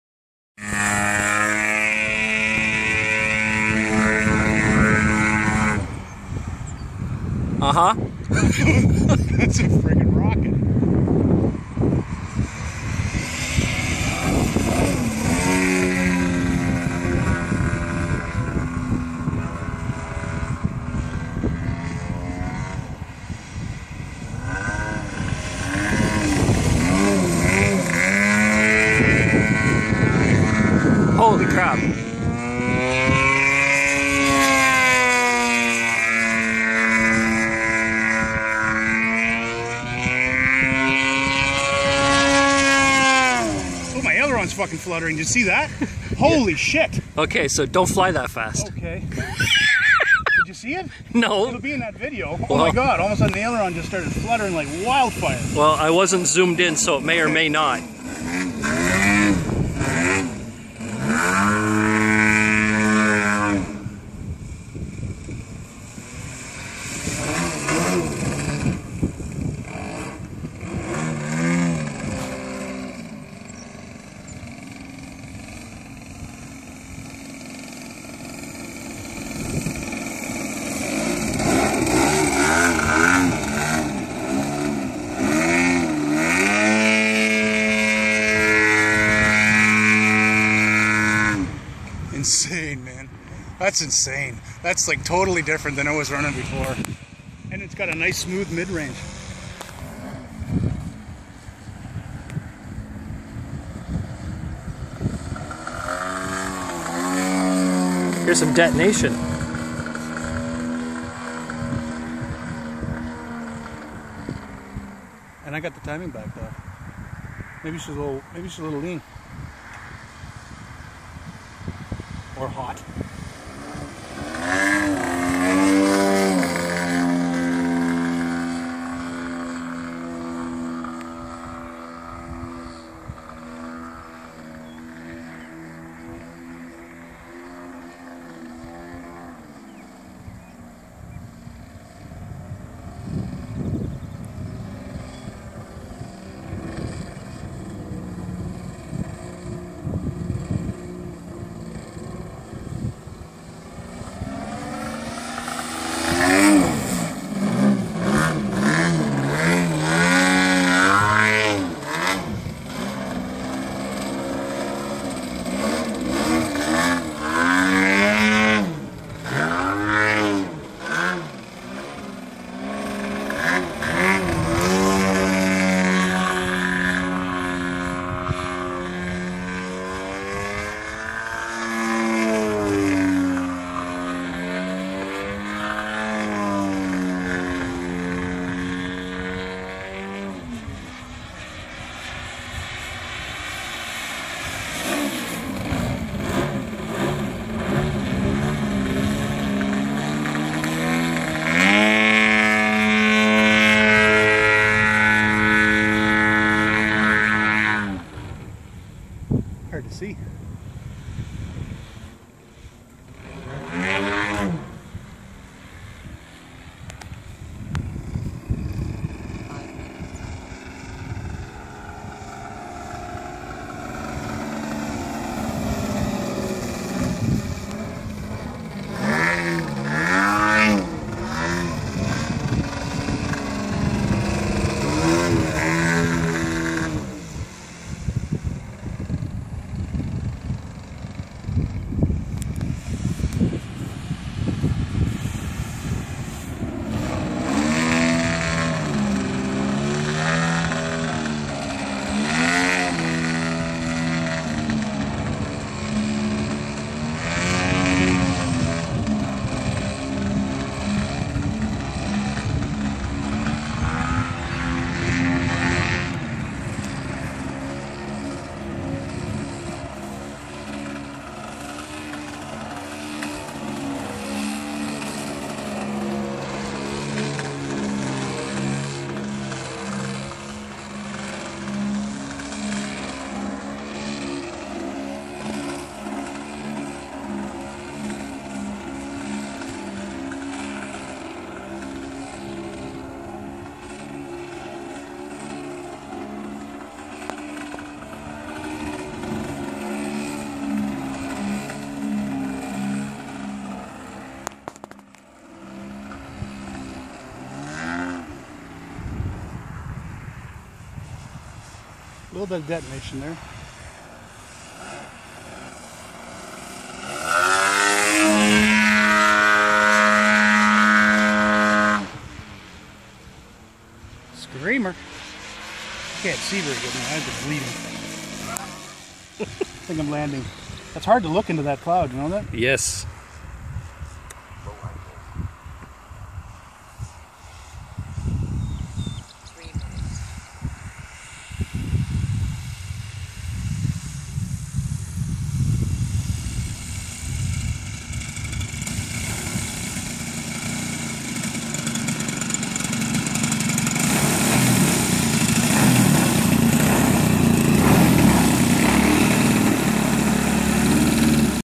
3DHS 103 inch Extra 330
3DHS 103 inch Extra 330 with a piped zdz 90 spinning a Falcon 28 X 9.5 prop